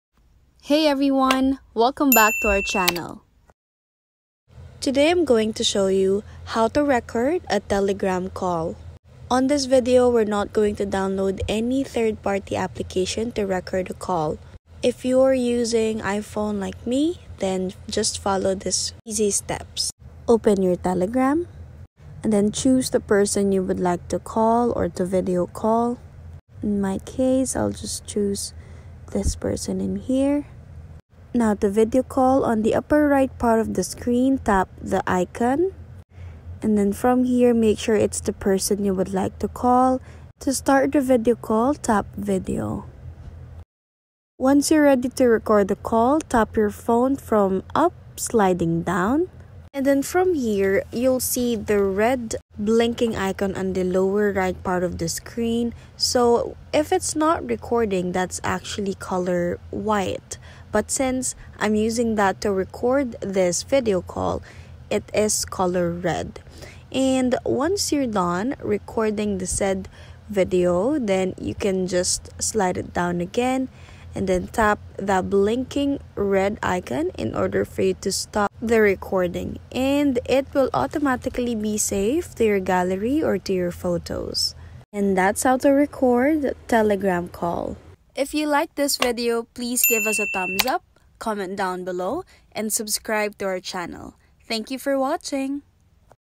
How To Record Telegram Call (EASY!)
How To Record Telegram Call. It’s a very easy tutorial, I will explain everything to you step by step.